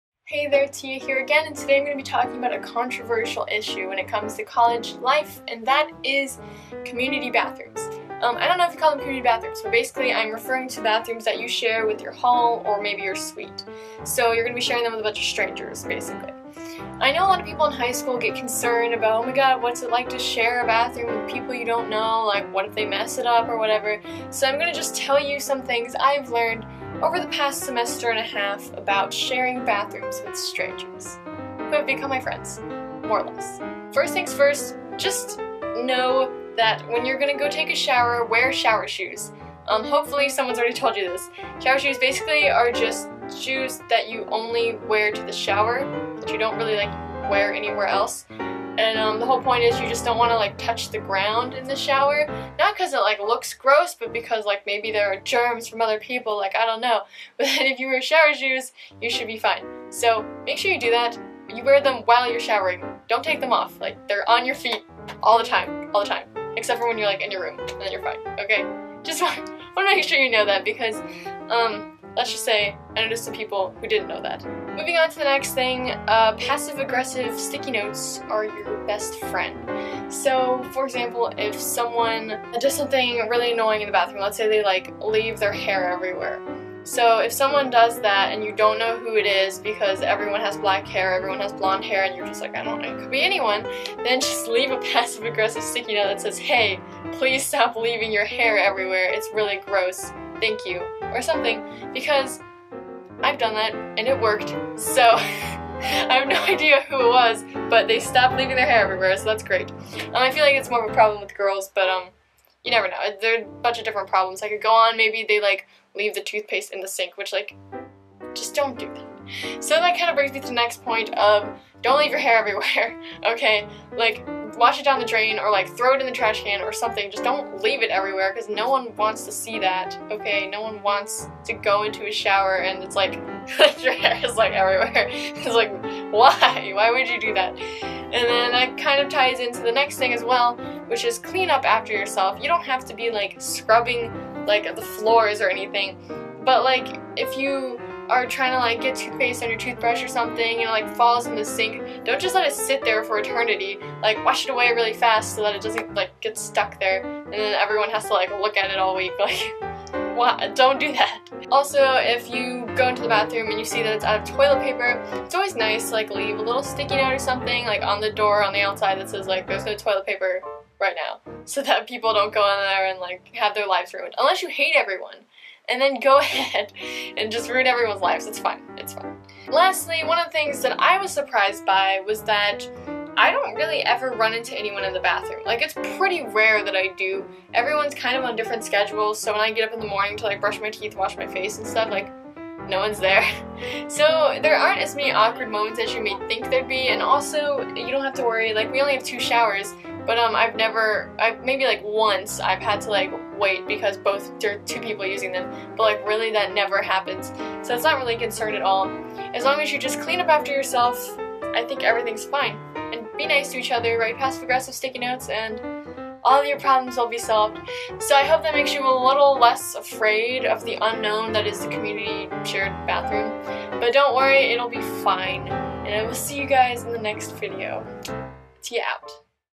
Sharing a Bathroom in College
Tia Lewis from Cornell University on sharing a bathroom in college